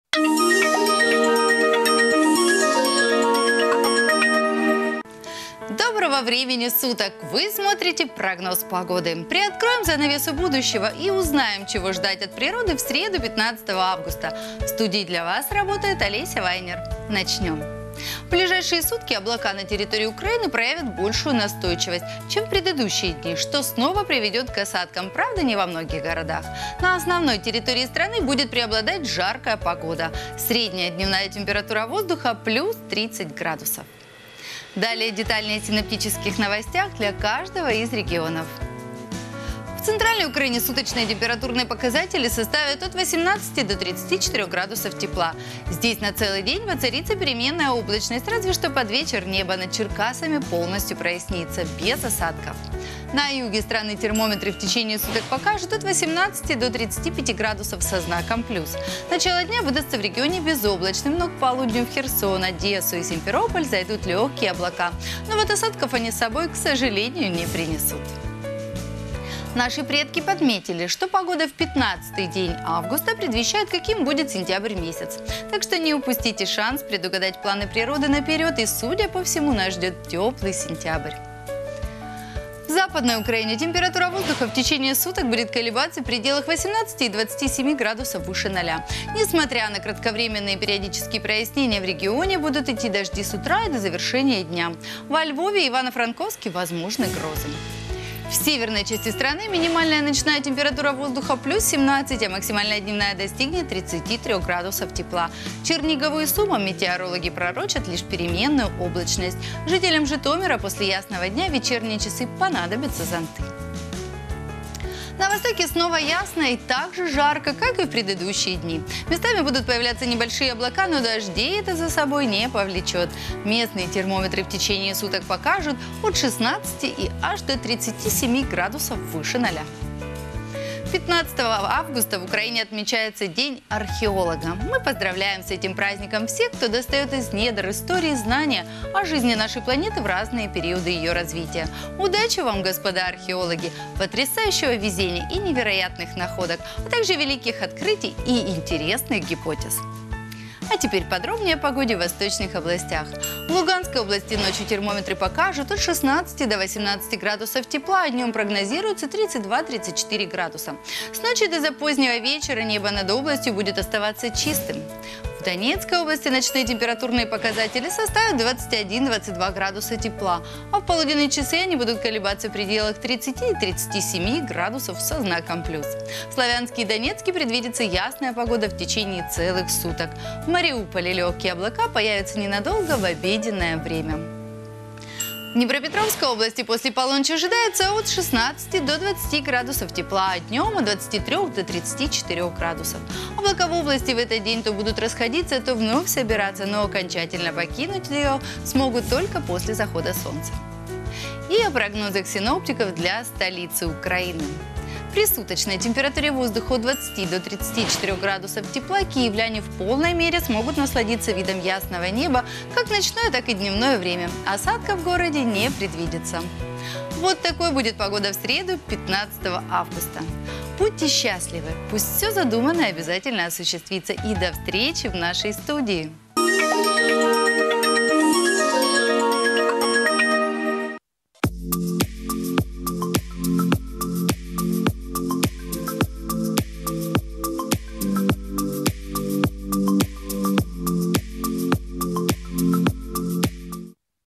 Погода 15.08.2018
Облака в Днепропетровской области в этот день то будут расходиться, то вновь собираться, покинут ее только после захода солнца.